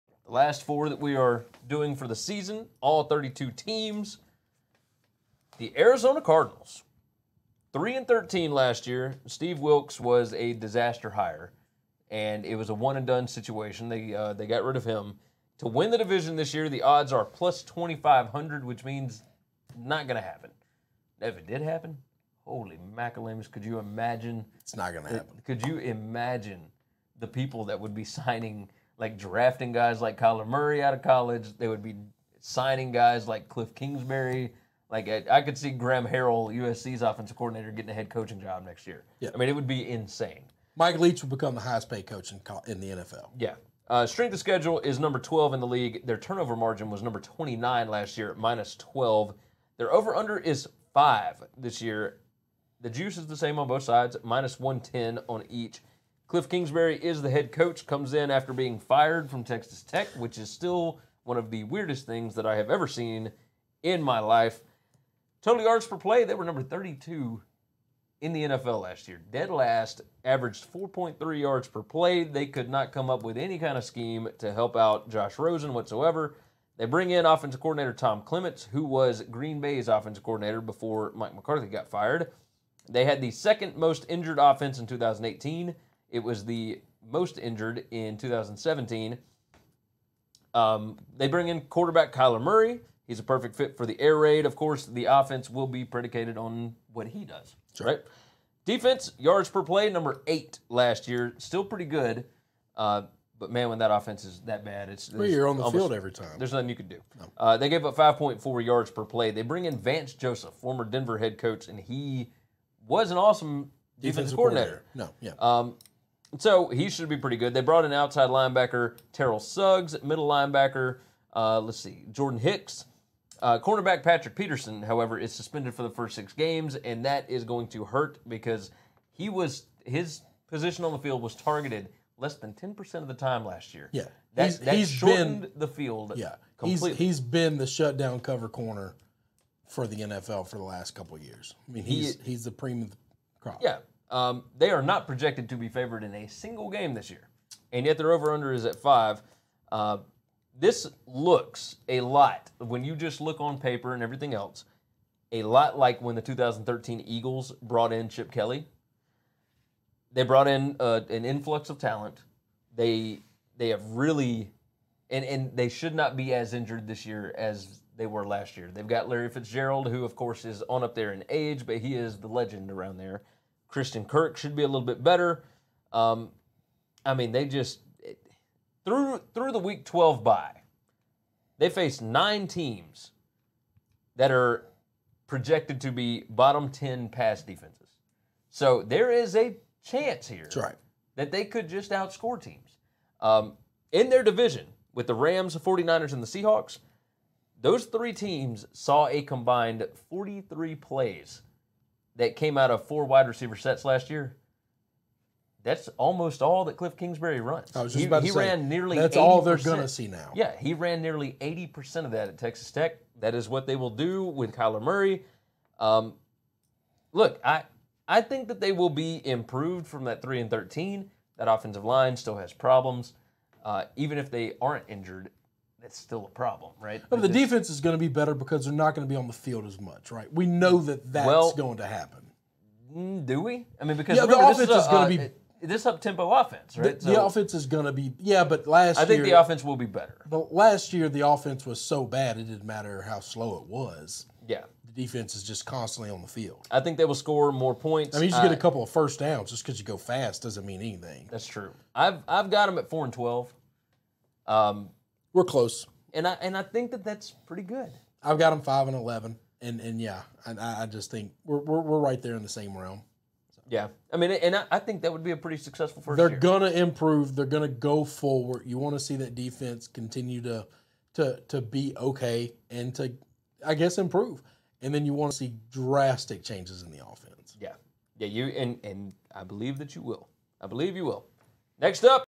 WCE: Arizona Cardinals 2019 NFL Preview & Predictions
Gary and Chris preview the Arizona Cardinals, and give their record predictions for the 2019 NFL season.